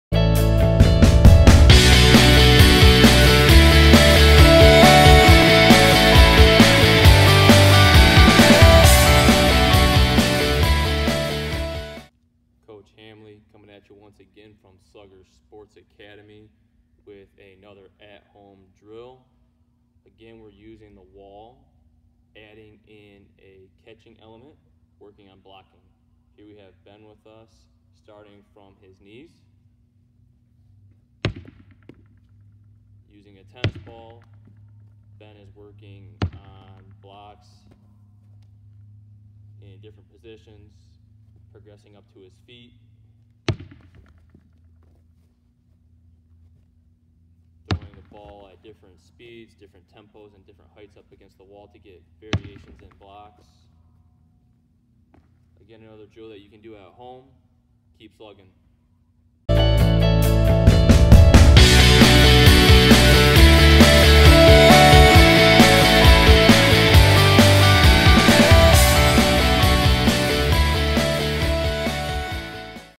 At Home Drill and Skill Wall Drill Catcher Blocking
Part of a series for athletes looking to sharpen their skills at home.  Coach Hamly is the current hitting coach at Calvin University, NCAA DIII, and an instructor at Sluggers.  Be sure to check-in every Tuesday and Thursday for the latest content.

for more information about us. Remember,